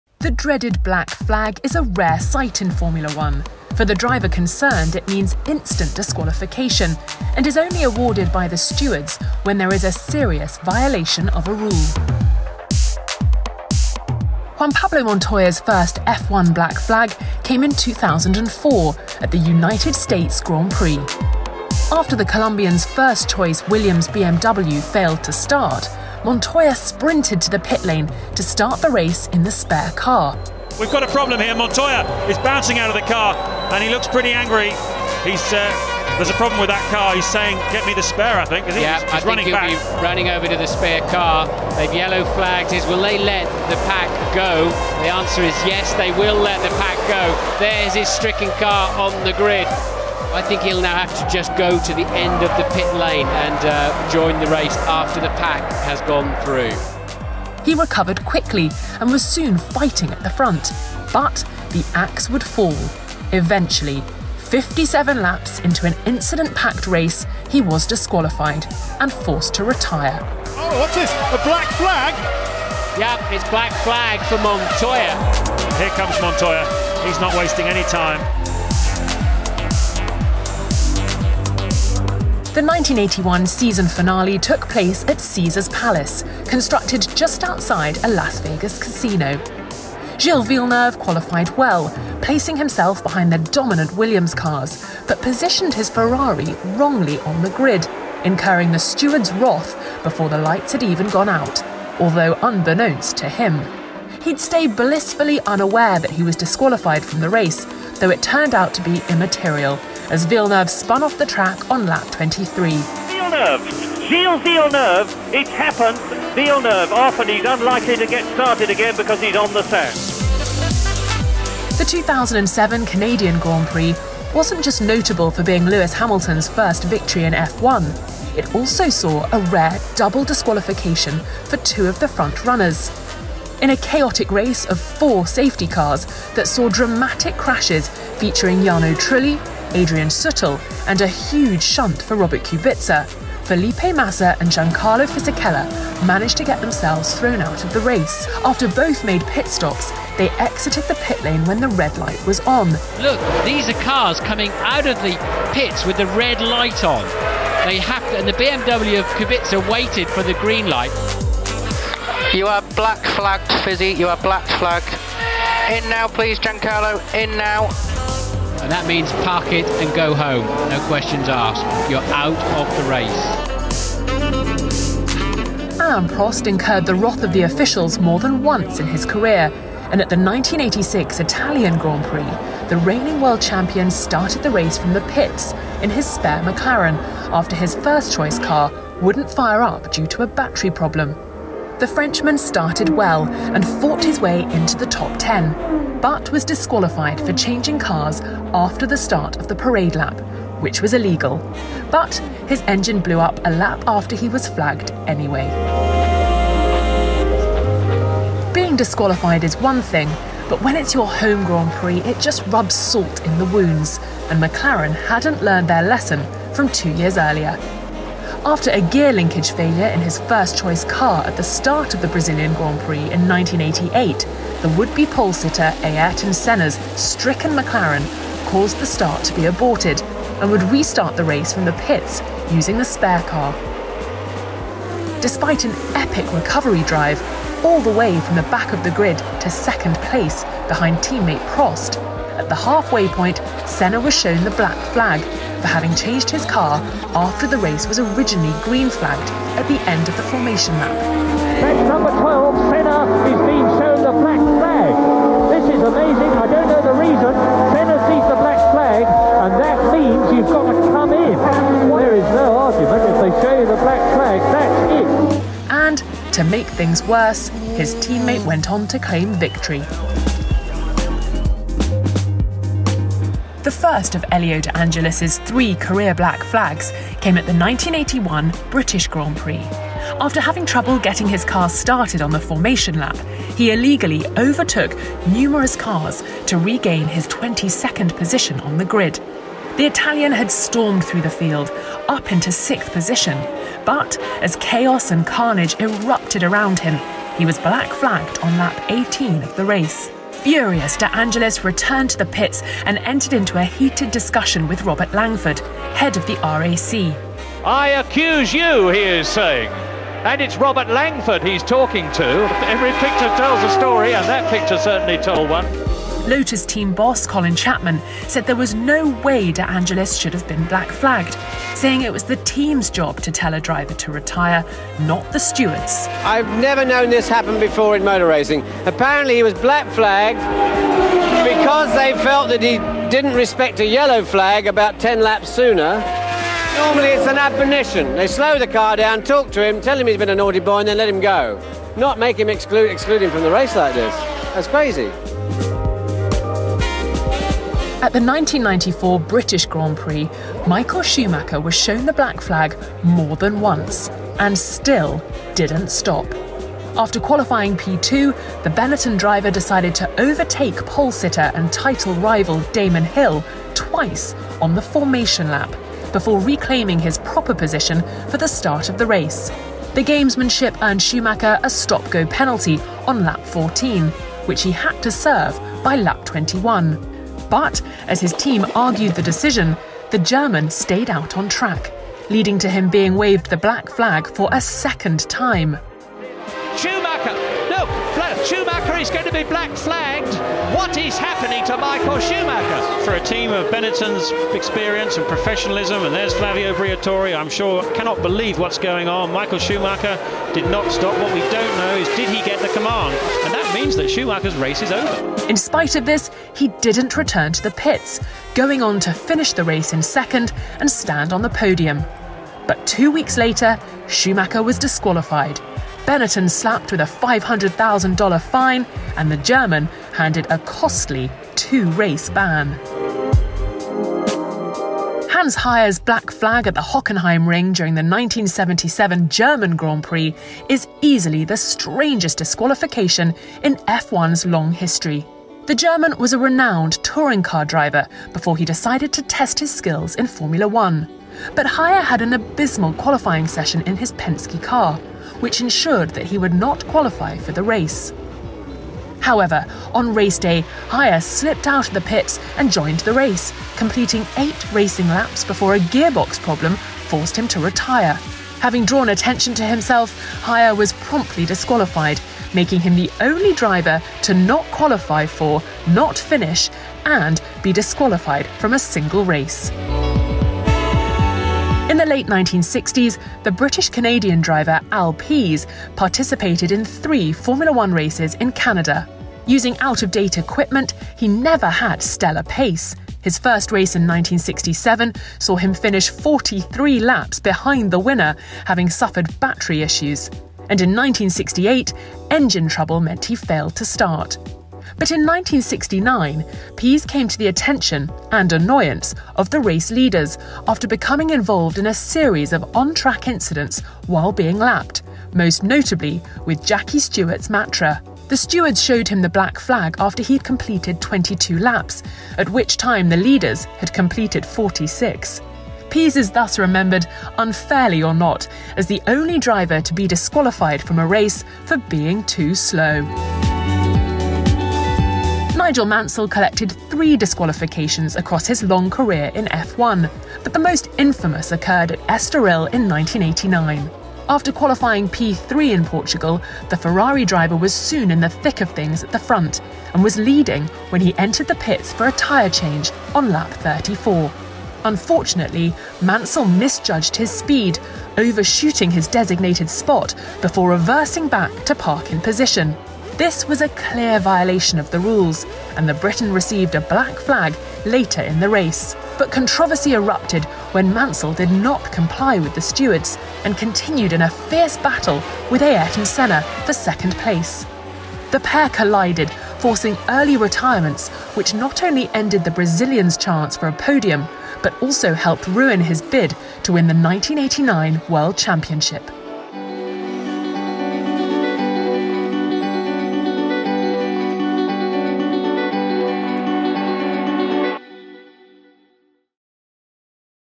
Top 10 Infamous Black Flag Moments
Driver disqualifications are rare but they are always dramatic - we take a look back at some notorious examples.